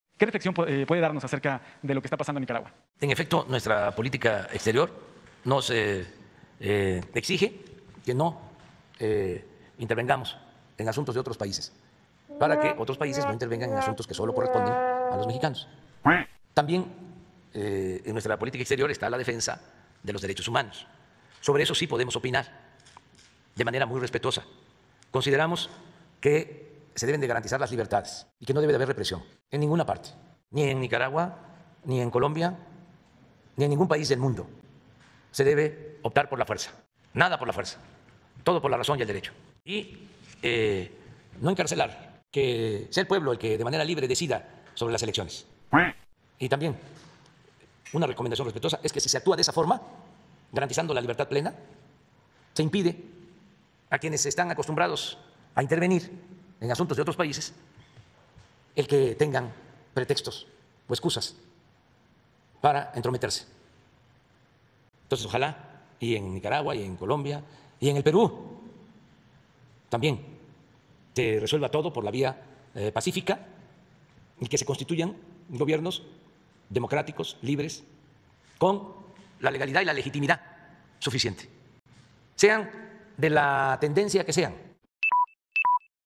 Sobre Nicaragua, nuestra política exterior nos exige que no intervengamos en asuntos de otros países
Sobre Nicaragua: Nuestra política exterior nos exige que no intervengamos en asuntos de otros países, para que otros países no intervengan en asuntos que solo corresponden a los mexicanos 🦆